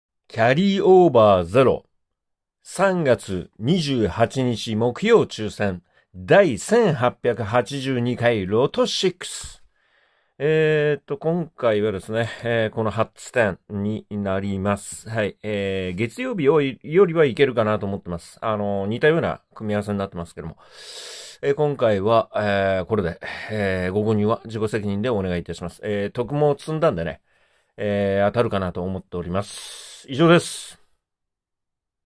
第1882回ロト6【これ購入済‼】8点!（2024/03/28木曜抽選）本年、25回目の抽選
当買い目に関して、
自己の責任の上で、

購入の検討をして
もらえればと思います。

当人は、
リアルに購入済！

【JRAの星矢のX(旧Twitter)】

【JRAの星矢のスレッズ】